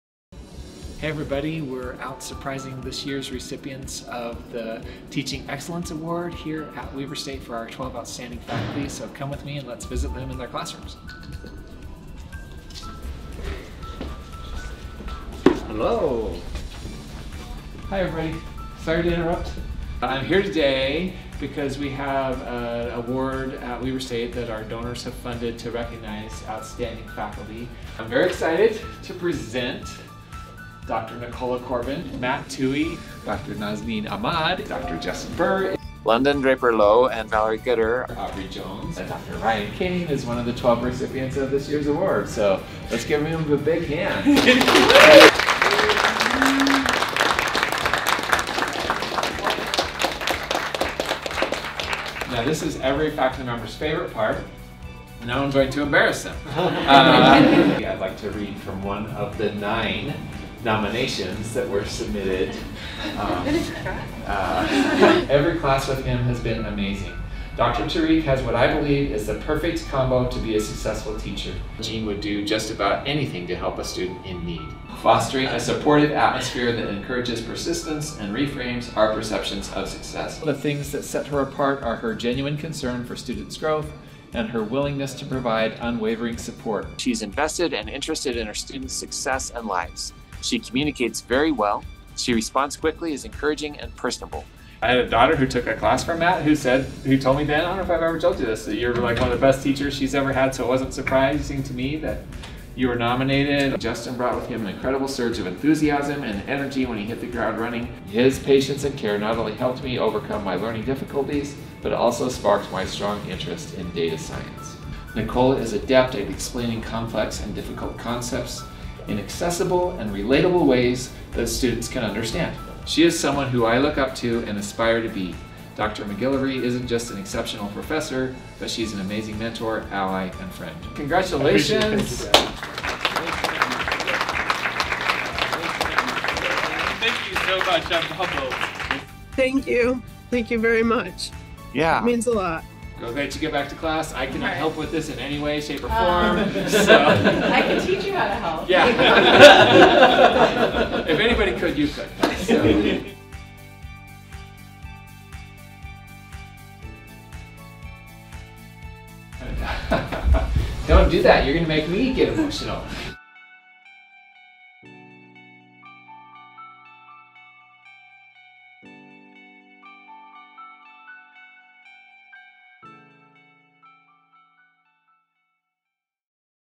Celebrating Weber State's 2024 Teaching Excellence Award Winners
Every year, Weber State University recognizes 12 professors with the Presidential Teaching Excellence Award. This February, University President Brad Mortensen surprised the 2024 award winners with the news in their classrooms and online.

Congratulations to the 2024 Award Winners:
Nazneen Ahmad
Tariq Arif
Justin Burr
Ryan Cain
Nicola Corbin
London Draper Lowe (Annie Taylor Dee Teaching Excellence Award)
Valerie Gooder (Annie Taylor Dee Teaching Excellence Award)
Bridget Hilbig
Aubrey Jones
Shannon McGillivray
Jean Norman
Matthew Thue